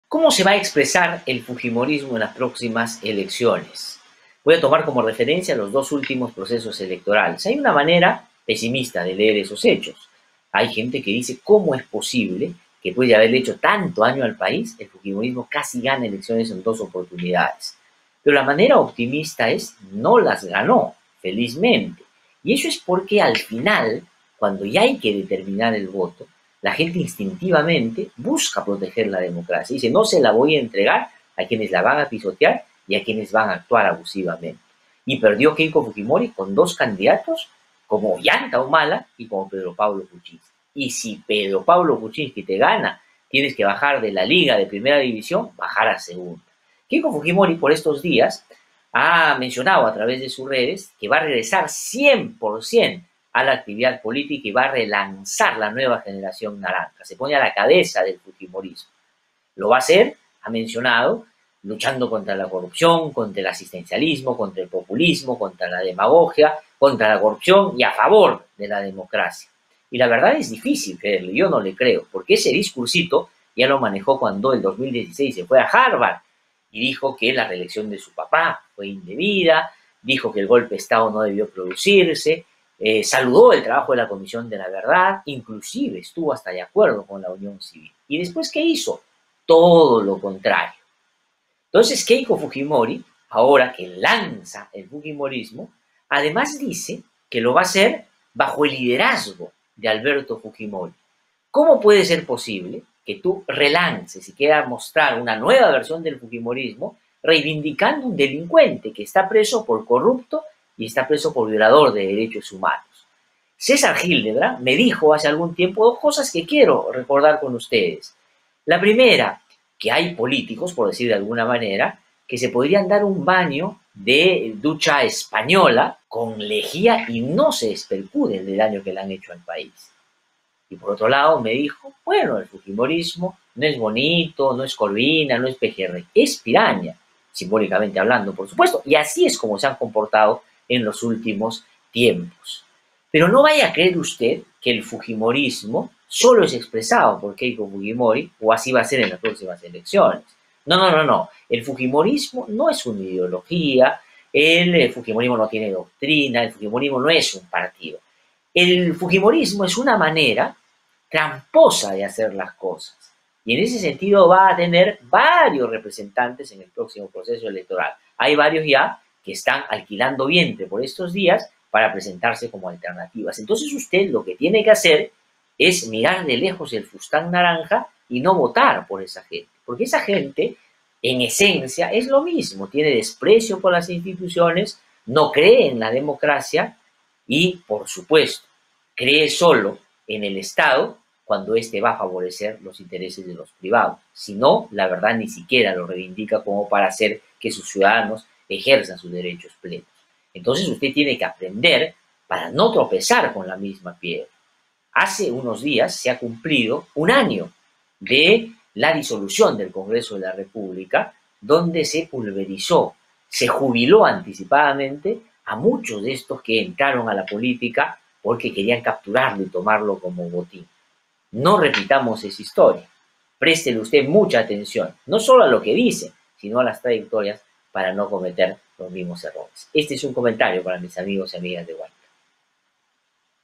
¿Quiénes serán el fujimorismo en las elecciones?
El fujimorismo vuelve de la mano de Keiko, quien también reivindica a su padre Alberto Fujimori. Otros personajes que los representan también intentarán llegar al poder.
Glatzer Tuesta de Ideeleradio te lo explica en 3Minutos